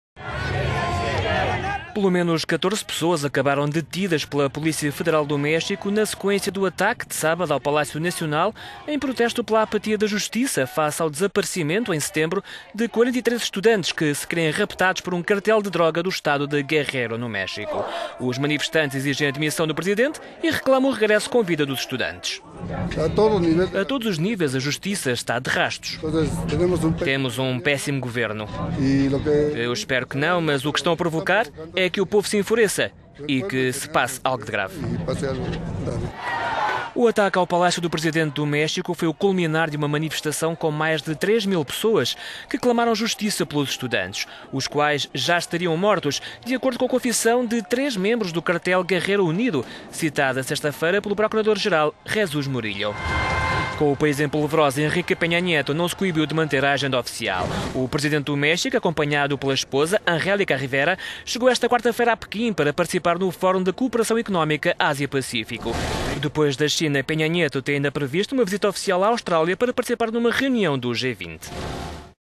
México: Polícia detém e liberta 15 pessoas após ataque ao Palácio Nacional
Pelo menos 15 pessoas acabaram detidas e interrogadas pela Polícia Federal do México na sequência do ataque de sábado à noite ao Palácio Nacional, no Distrito Federal mexicano. O ataque ao edifício habitualmente usado pelo Presidente Enrique Peña Nieto foi culminar da massiva manifestação com mais de 3 mil pessoas que protestaram pela apatia do governo e da justiça face ao desaparecimento de 43 estudantes no final de setembro no Estado mexicano de Guerrero.